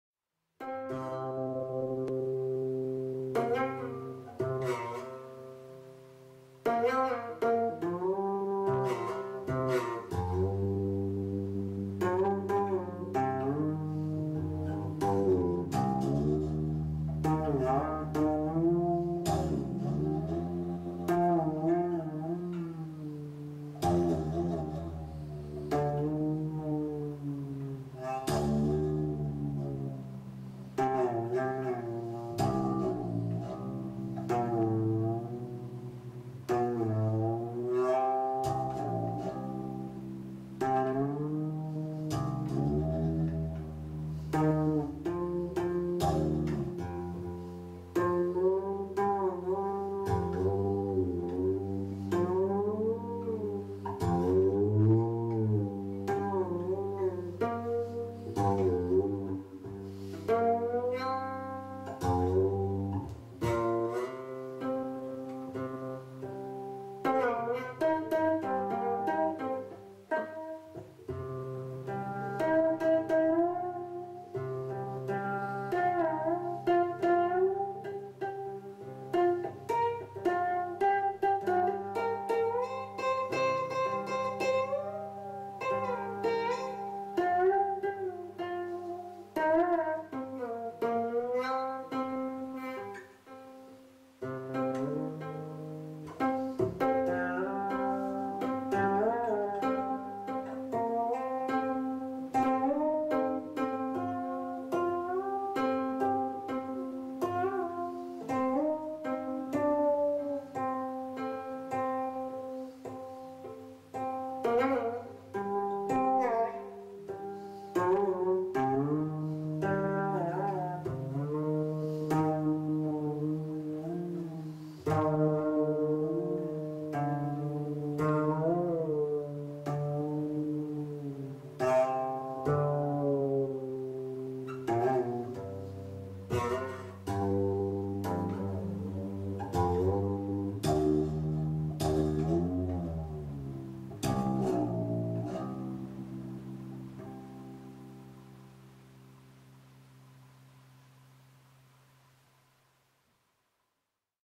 Daniel Schell plays the Boditar #3
Player: Daniel Schell
The Boditar is a fretless Tap-guitar builded by Siggi Abramzik

Brussels 2011